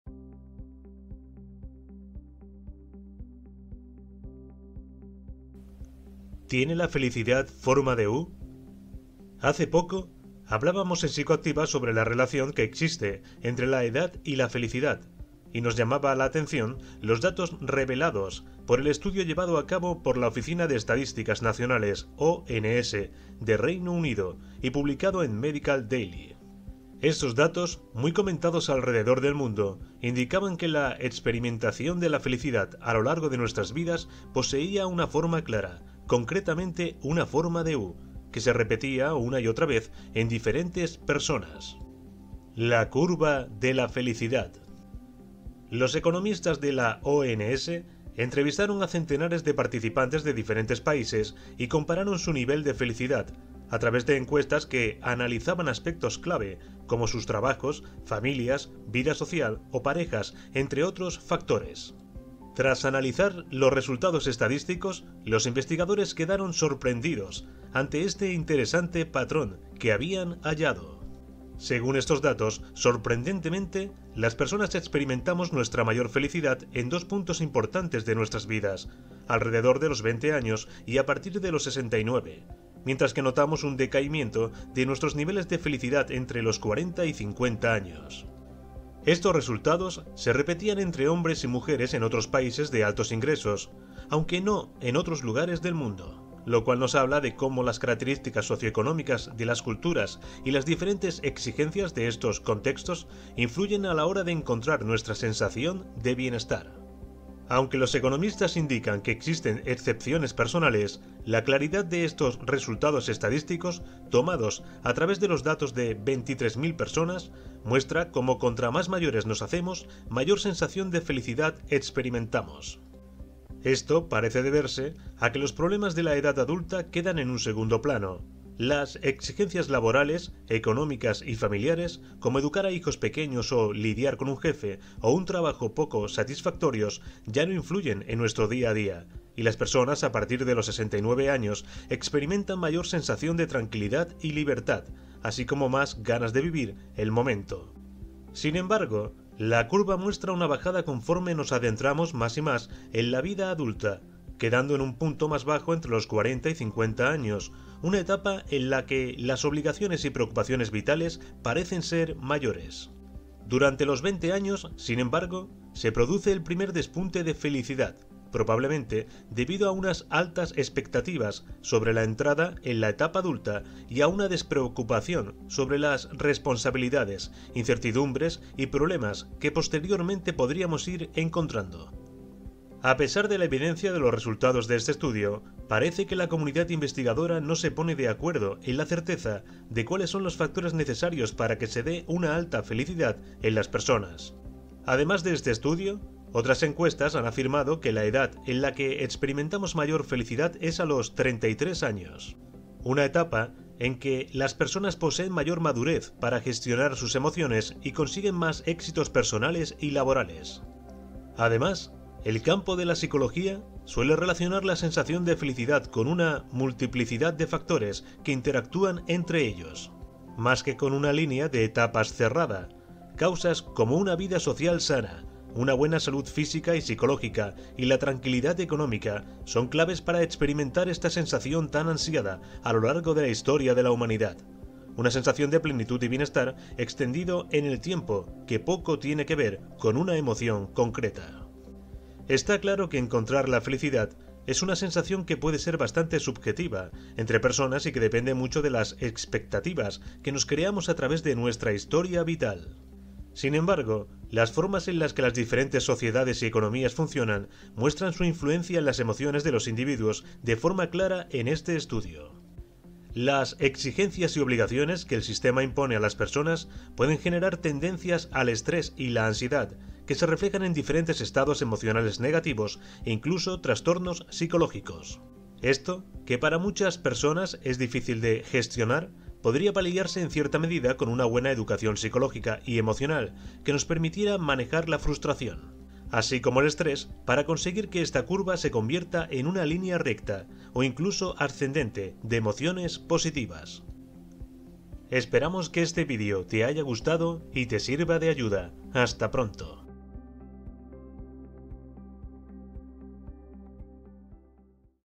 La felicidad tiene forma de U
Según algunos estudios recientes, el nivel de sensación de felicidad a lo largo de nuestra vida tiene una curiosa forma de U, ¿quieres saber por qué?